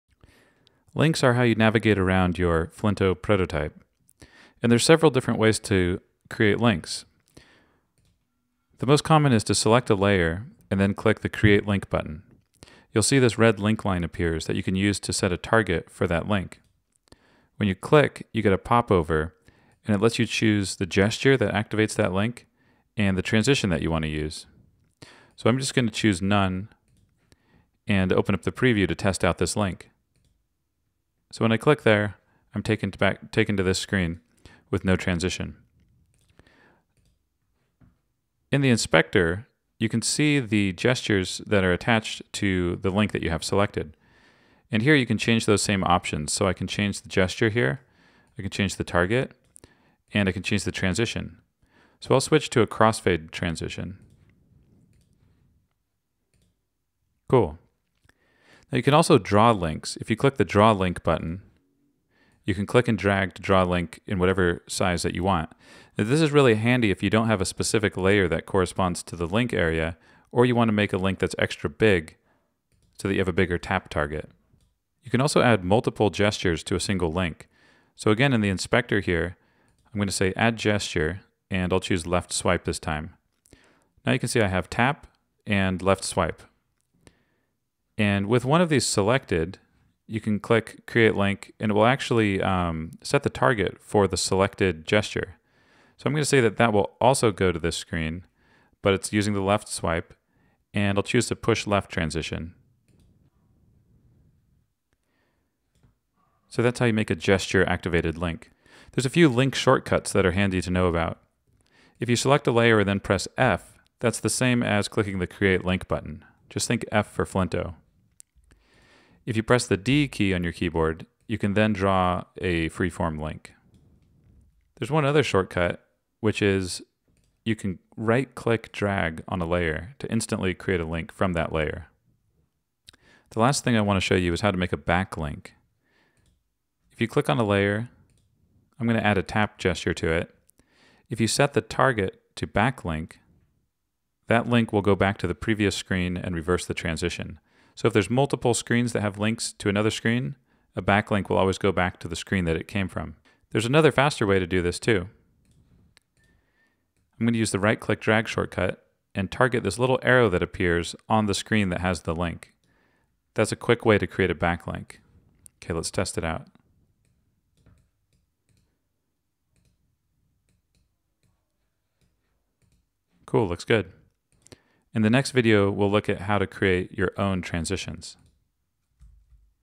Creating Links
Add interactivity to your prototype by creating links between screens. Flinto for Mac makes it easy!